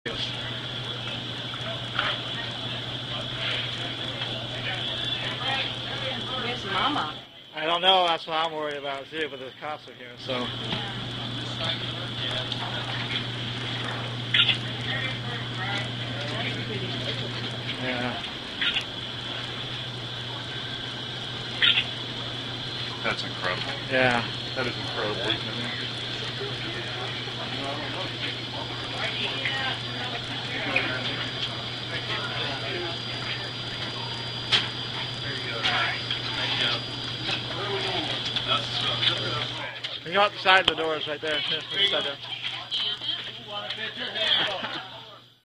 Raw Video: Bear Caught in Alaska Grocery Store
Some shoppers in Ketchikan, Alaska got treated to that state's version of a bear market last weekend. A frightened black bear cub got into a grocery store and made a mess in the produce section.  (Oct. 19)